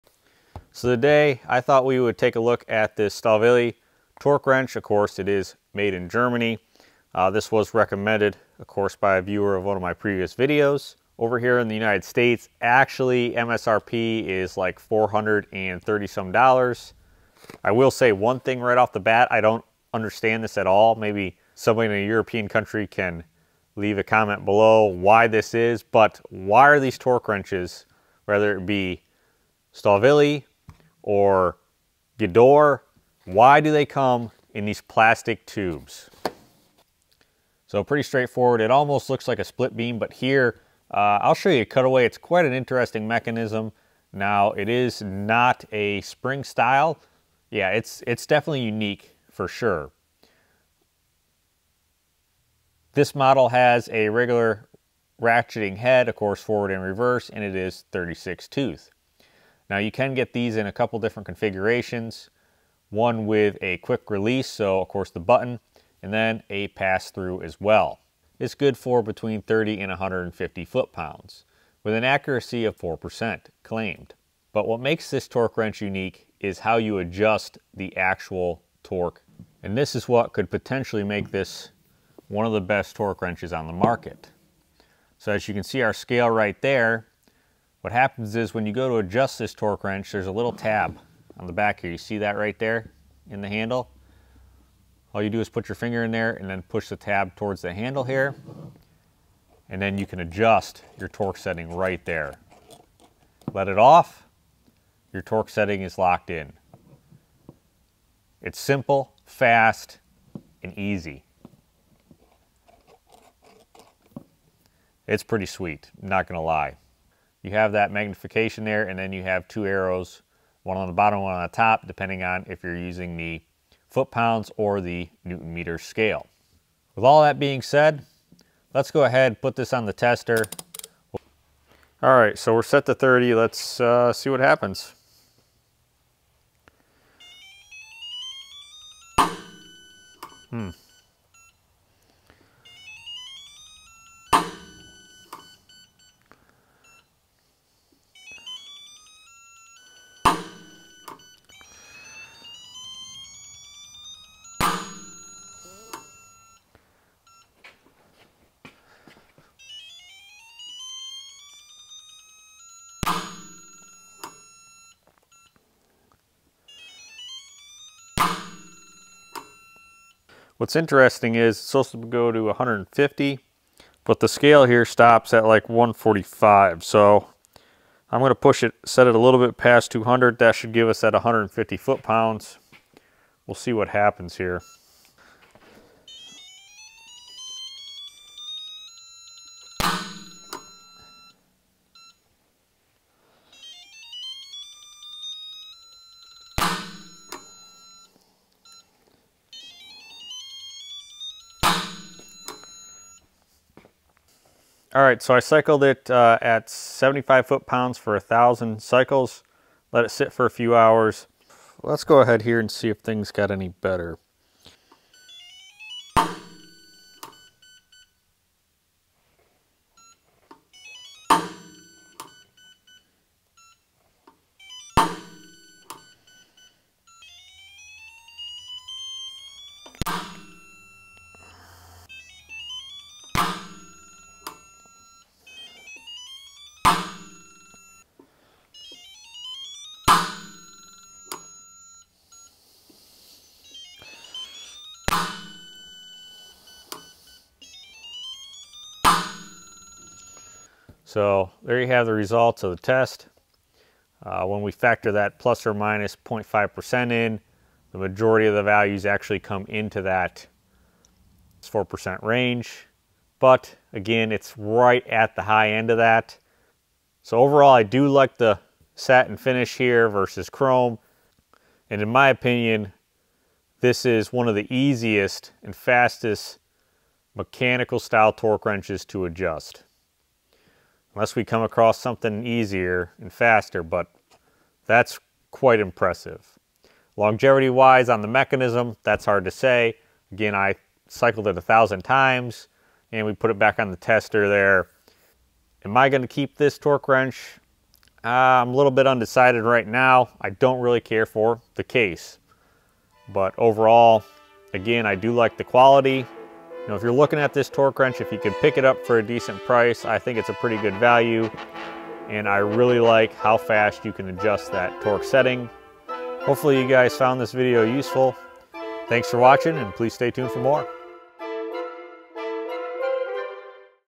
Fastest Torque Adjustment, On A Mechanical Torque Wrench? Made In Germany Stahlwille
Made In Germany Stahlwille 1/2 Torque Wrench

Amazon Link

Thank You! for supporting the channel, this helps in me providing new content each week!